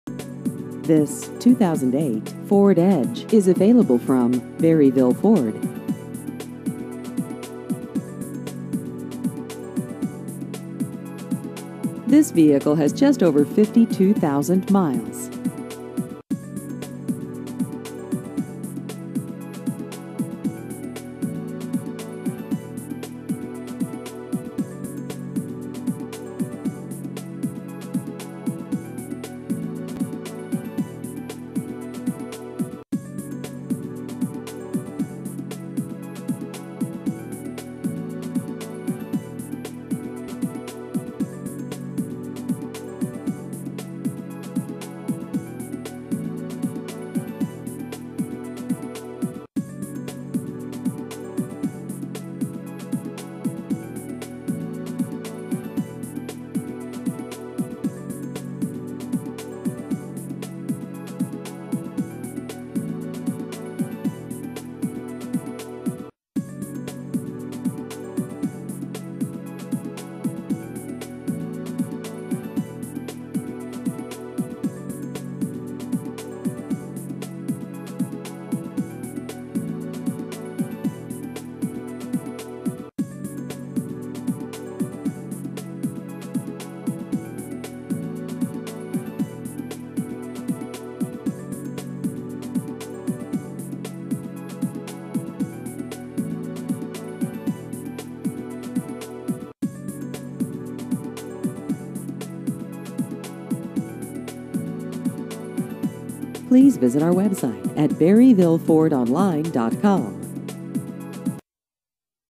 2008 Ford Edge Used Cars Berryville AR
This 2008 Ford Edge is available from Berryville Ford.